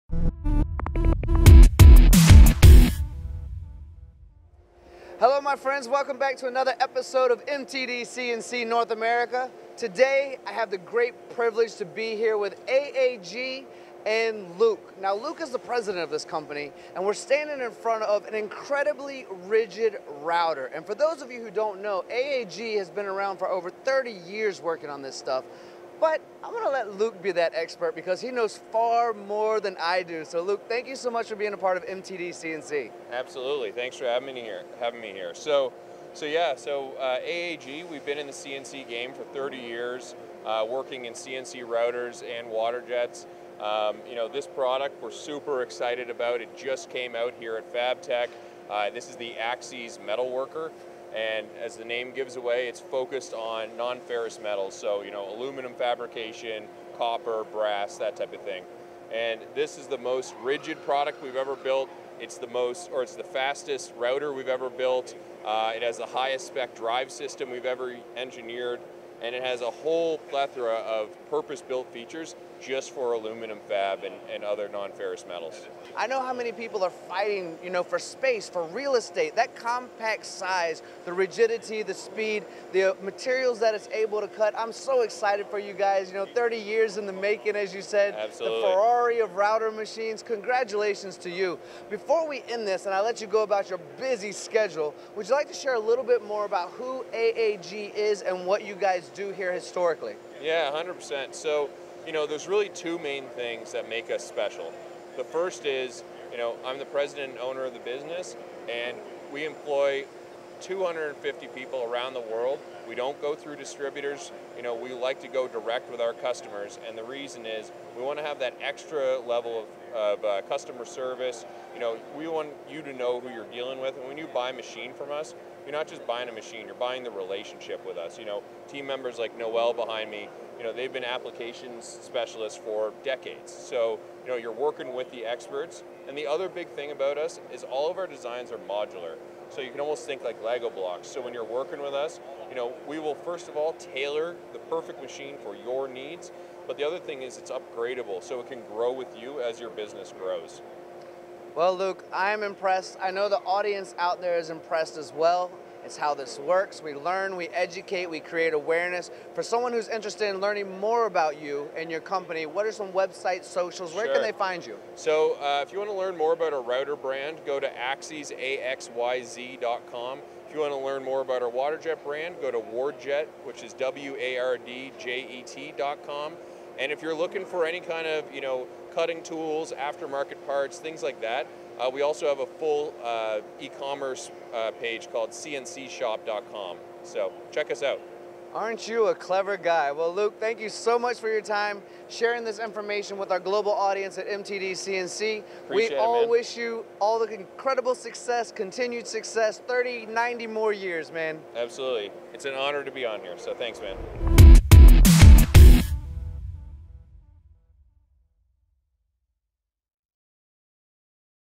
MTD-CNC's Tony Gunn Interviews AAG CEO on METALWorker
The All-New METALWORKER CNC Router provides Aluminum fabricators with the fastest, most rigid construction and design AXYZ has ever built. In this video from FABTECH21, Luke Hansen-MacDonald (President, AAG) chats with Tony Gunn (MTDCNC.GLOBAL) about the plethora of purpose-built features that make the METALWORKER one of the best-tailored machines that meet the specific business needs of the aluminum, metal, and other non-ferrous materials community.  for more information.